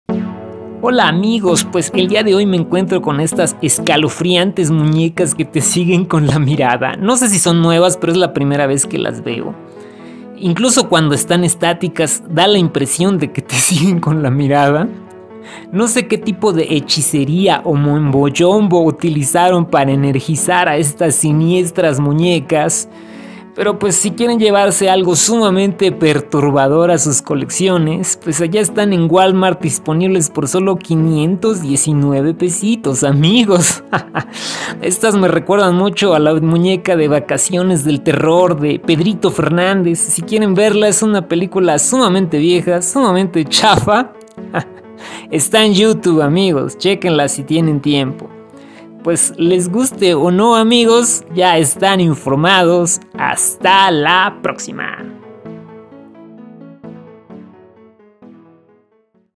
Éstas muñecas me dieron miedo... Unique Eyes
Hola amigos!! hoy ví éstas muñecas dignas de una película de terror...  Salu2